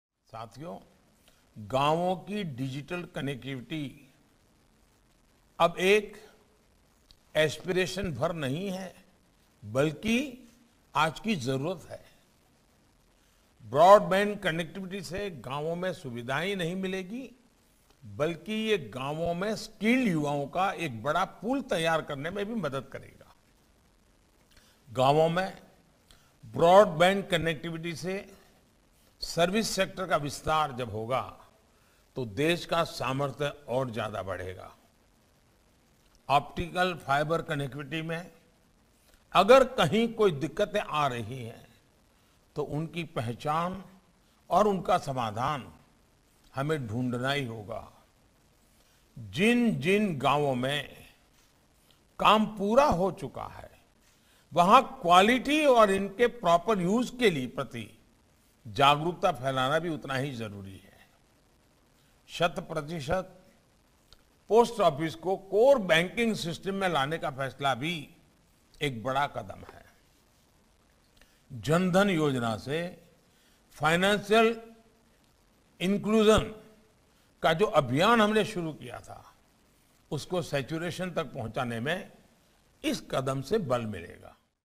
गांवों की डिजिटल कनेक्टिविटी अब एक aspiration भर नहीं है, बल्कि आज की जरूरत है
ब्रॉडबैंड कनेक्टिविटी से गांवों में सुविधाएं ही नहीं मिलेंगी, बल्कि ये गांवों में स्किल्ड युवाओं का एक बड़ा पूल तैयार करने में भी मदद करेगा।

- पीएम श्री नरेन्द्र मोदी